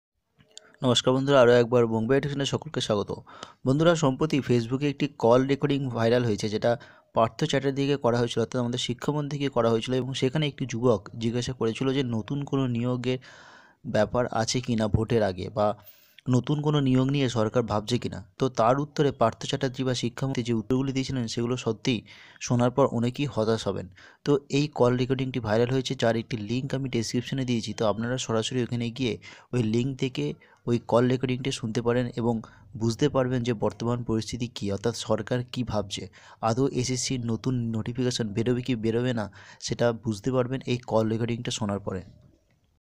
New SSC Notification|ParthoChattopadhyay Call Recording|শিক্ষক নিয়োগ নিয়ে একি বললেন শিক্ষা মন্ত্রী
🔥 ফেসবুক থেকে পাওয়া এই কল রেকর্ডিং, যেখানে শিক্ষা মন্ত্রী বলছেন এখন নতুন করে শিক্ষক নিয়োগের কোন আশা নেই।
(এই রেকর্ডিং এর সত্যতা যাচাই করে নি Bongbio Education)